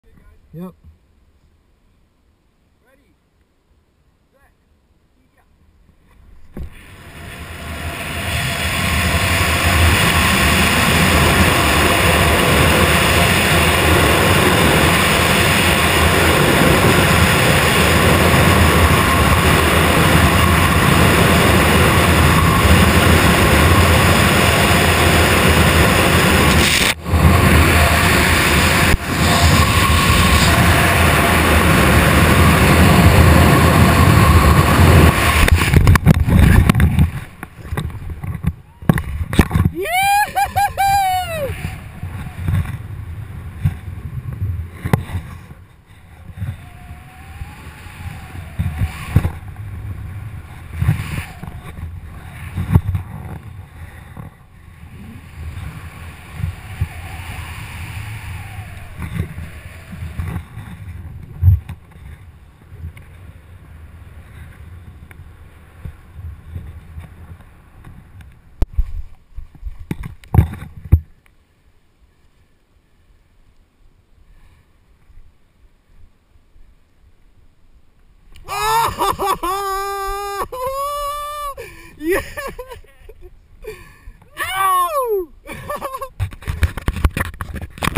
Wingsuit through waterfall in Lauterbrunnen
Wingsuit through the Aegerstanfall from Low Ultimate in Lauterbrunnen, Switzerland. Found myself high enough and with enough energy on this flight after many practice attempts. Flying the Vampire 3 from Phoenix Fly. High fives to my South American brothers.